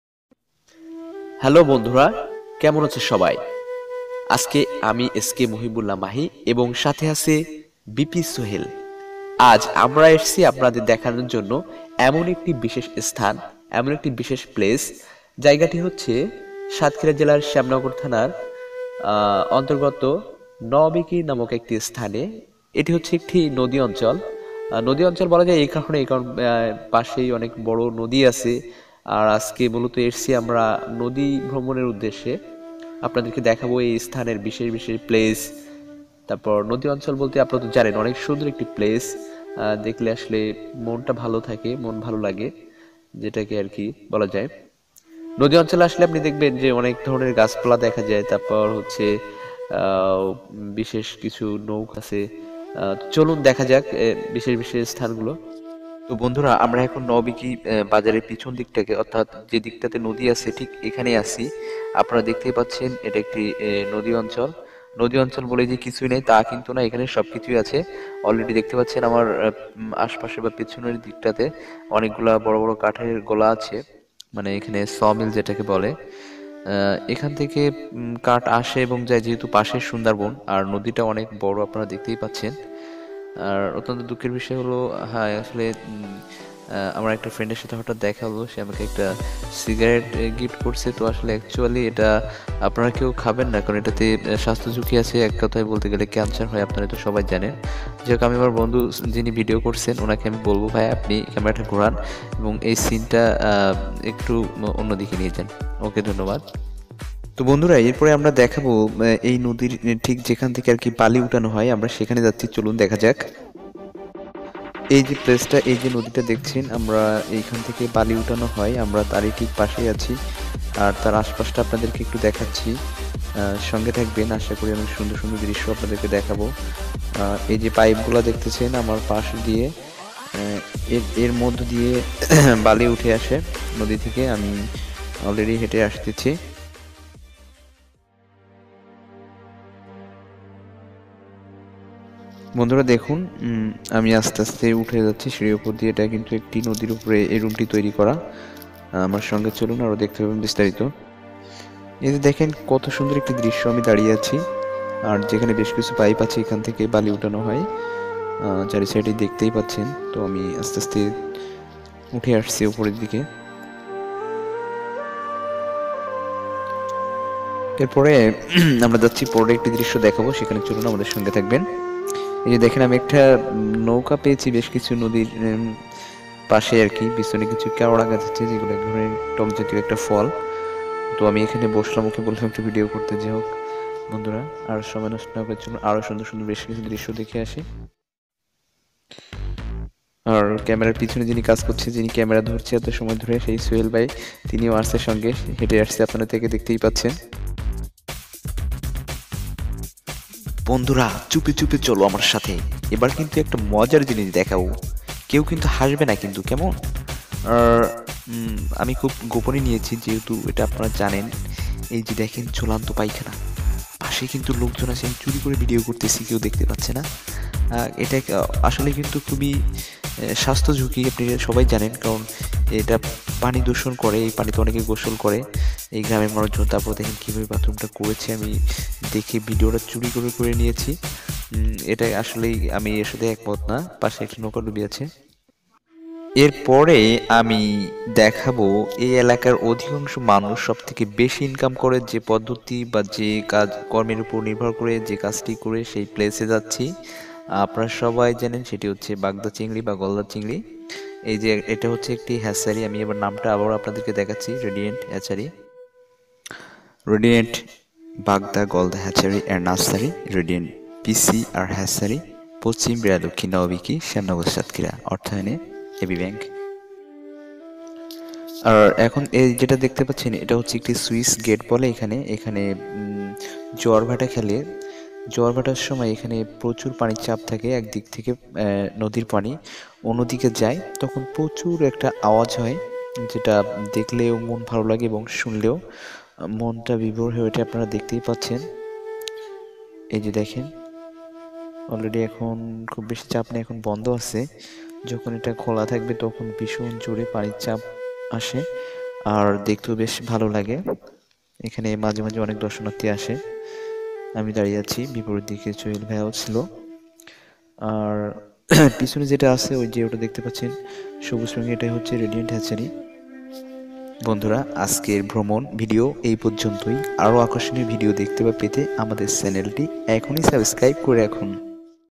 নদি অঞ্চল ভ্রমণ এর পূর্বে অসাধারণ দৃশ্য গুলি এক নজর দেখে নিন!!
এই ভিডিওটি মূলত ভ্রমণপিপাসু ব্যক্তিদের জন্য। কারণ এই ভিডিওতে অসাধারণ সিনসিনারি তুলে ধরা হয়েছে, বিশেষ করে নদী অঞ্চল এলাকা গুলো দেখানো হয়েছে। আজকের এই ভিডিওতে সাতক্ষীরা জেলার শ্যামনগর থানার অন্তগর্ত নওয়াবেকি নামক স্থানের নদি অঞ্চল  দেখানো হয়েছে এবং নদী অঞ্চলের আশপাশের পরিবেশ পরিস্থিতি গাছপালা ও। ভিডিওটি দেখে ভালো লাগলে লাইক কমেন্ট এবং শেয়ার করতে ভুলবেন না। ভিডিওটি ধারণ করেছে বিপি সোহেল এবং এডিট করেছে এসকে মহিবুল্লাহ মাহি দি মাদারস রেকর্ডিং হাউজ এন্ড ফেবারেবল এইচডি ভিডিও স্টুডিও চ্যানেলের সিইও।